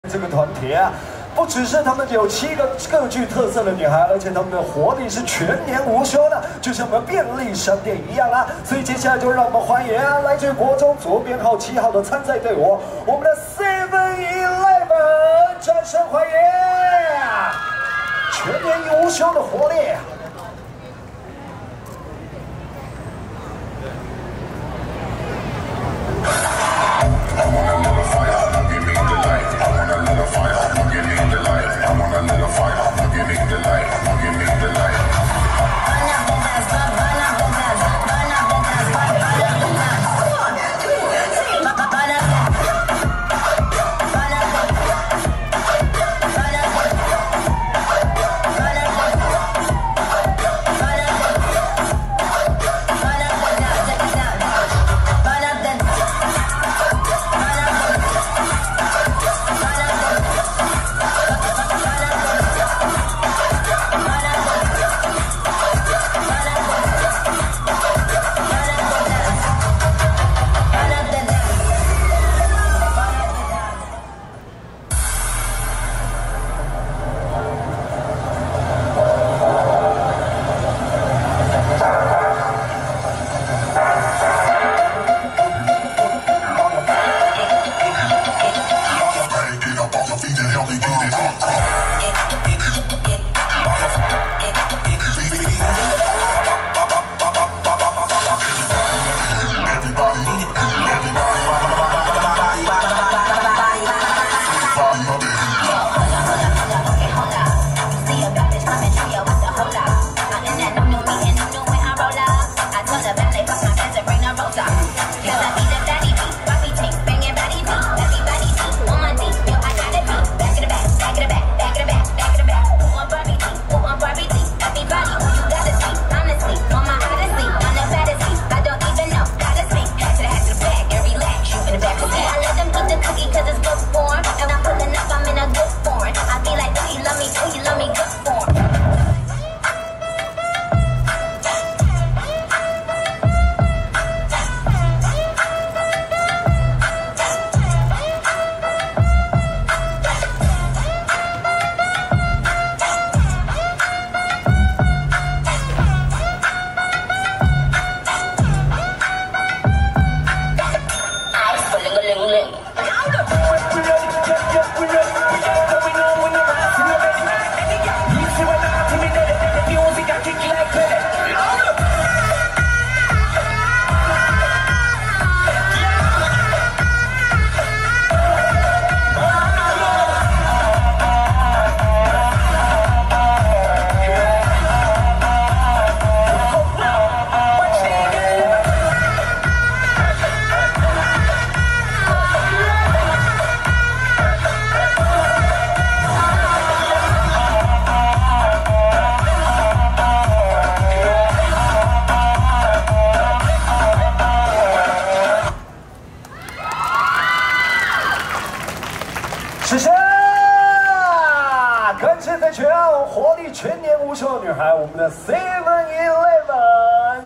109 全國中等學校熱舞大賽 HDC - 中區預賽 (國中組) 7-11
0:00 團體介紹 -  7-11
0:25 熱舞開始
1:18 第二段熱舞

HDC 2020 全國中等學校熱舞大賽  - 中區預賽
組別：國中組 - Seven-Eleven

主辦單位：中華民國高中體總
指導單位：教育部體育署